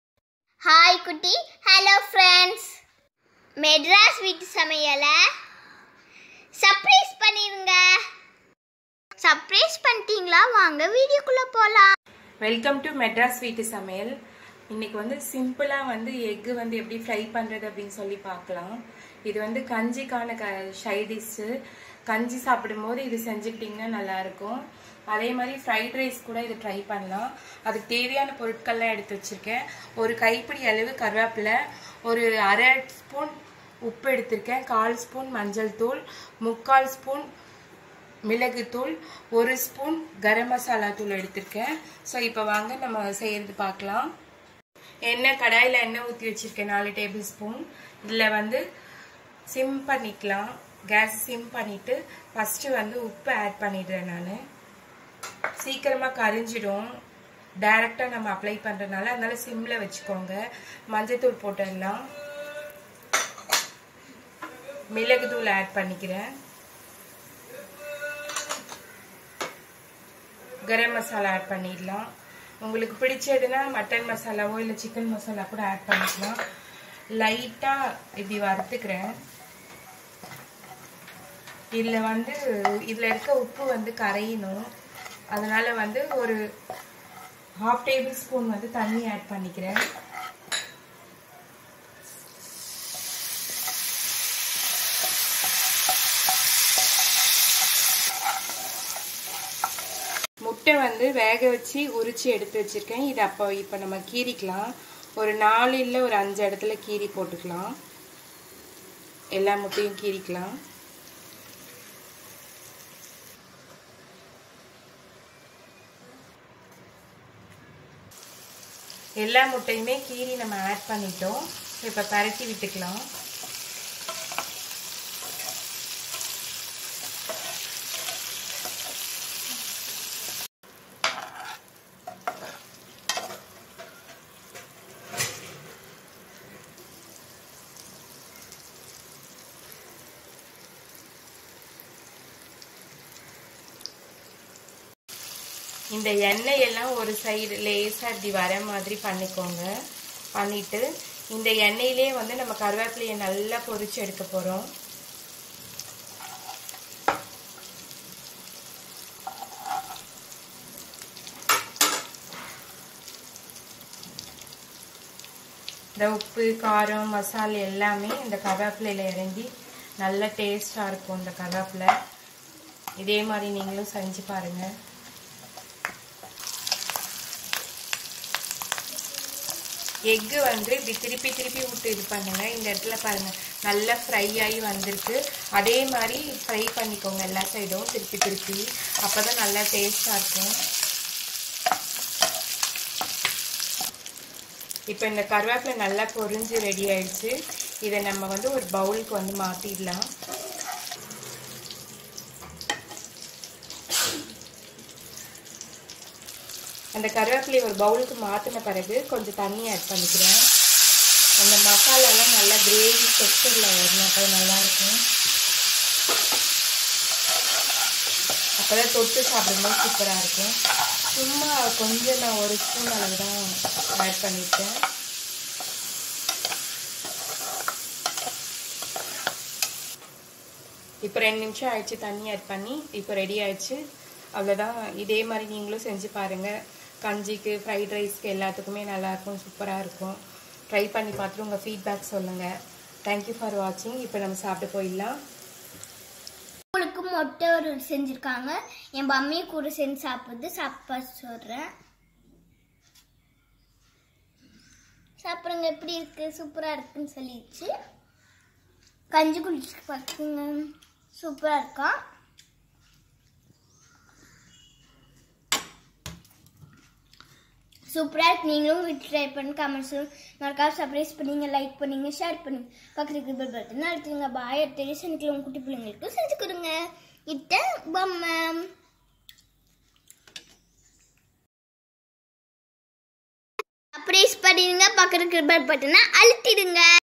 Healthy Porridge (kanci) Sidedish Full Egg Fry Recipe in Tamil/ முட்டை வறுவல்
In this video, we see how to make simply egg fry in tamil. Egg fry is a most
favour dish for maximum  People.

Like !!  Share !!  and support us !! 🙏🙏💖